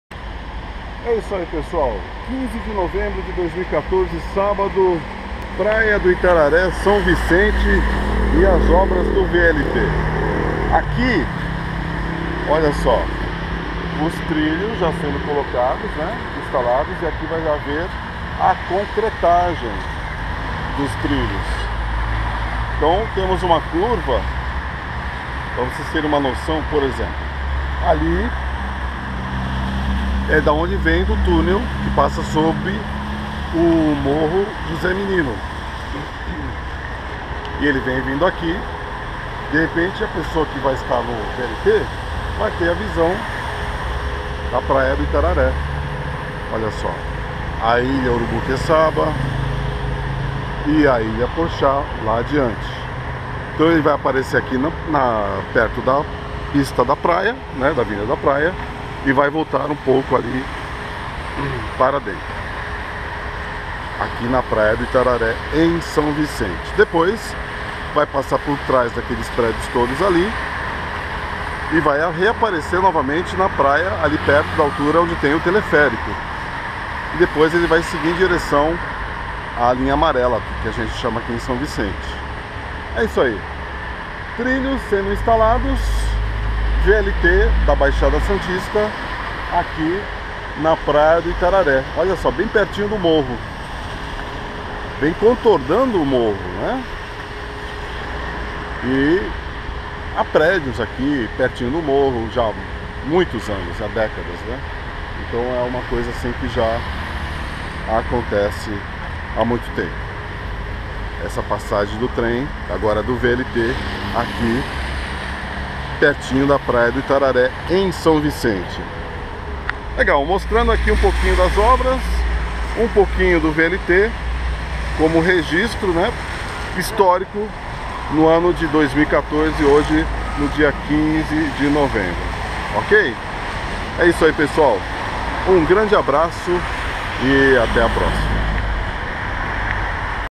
15.11.2014 VLT da Baixada Santista na Praia do Itararé
Trilhos do VLT sendo instalados próximo à avenida Padre Manoel da Nóbrega, na praia do Itararé em São Vicente